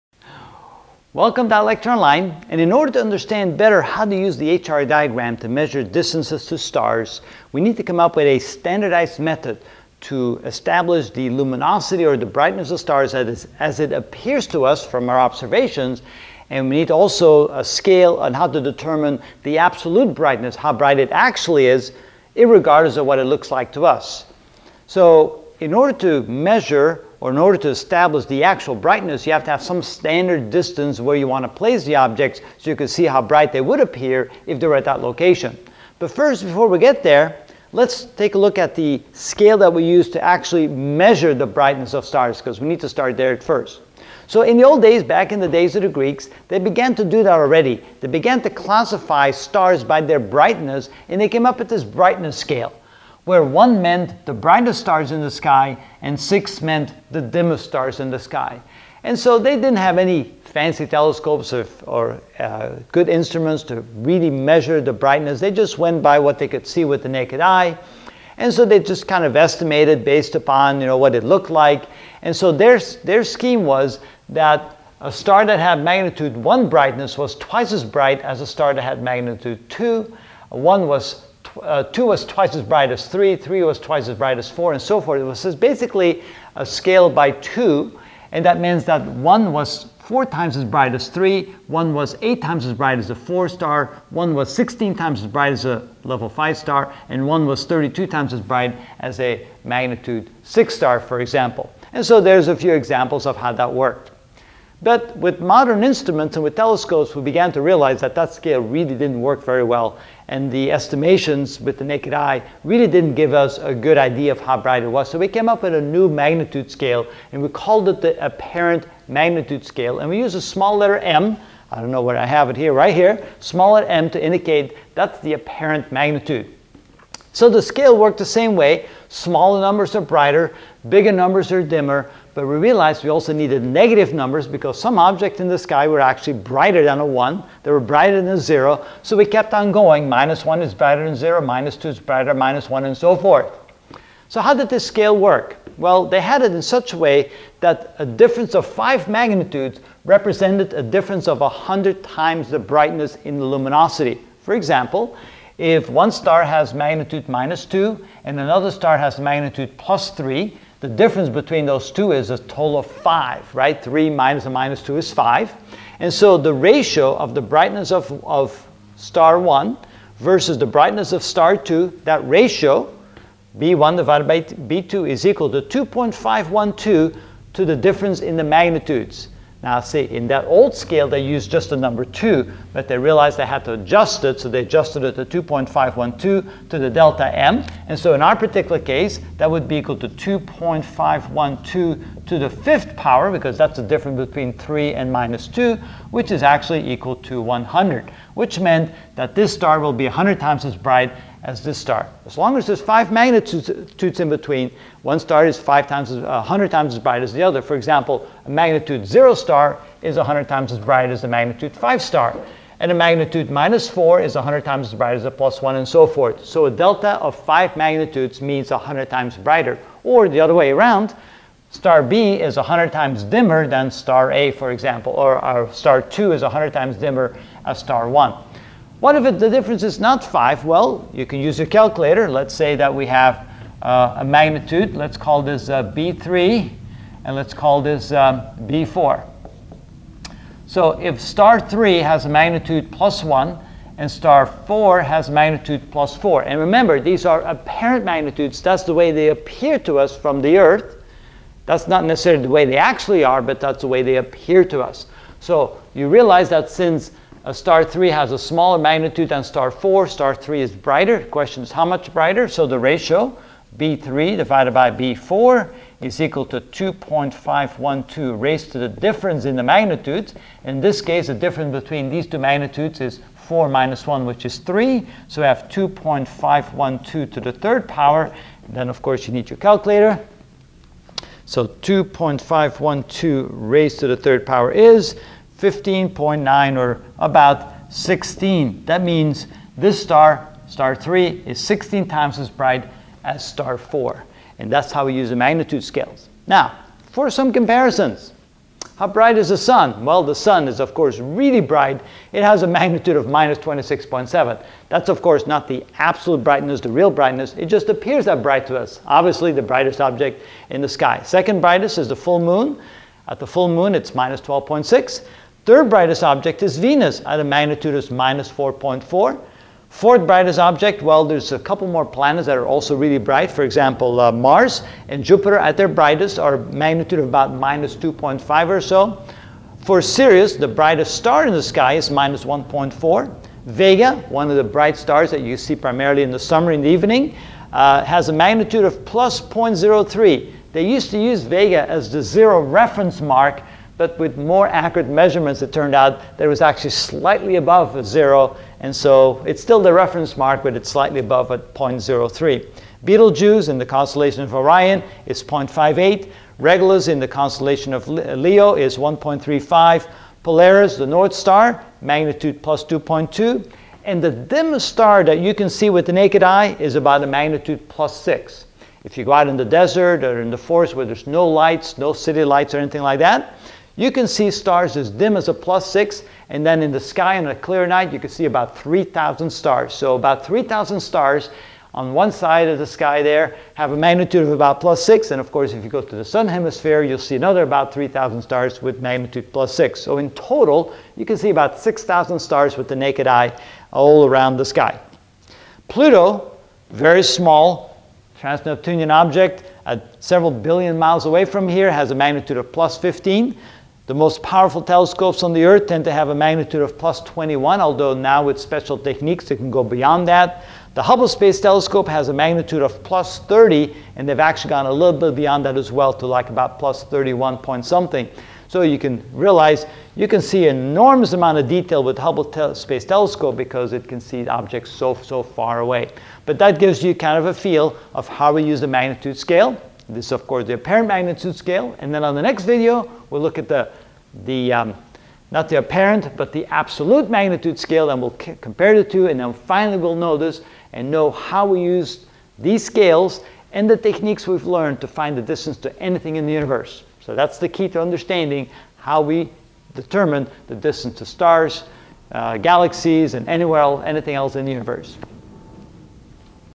Astronomy - Measuring Distance, Size, and Luminosity (17 of 30) Apparent Magnitude
In this video I will explain apparent magnitude (old scale and new scale).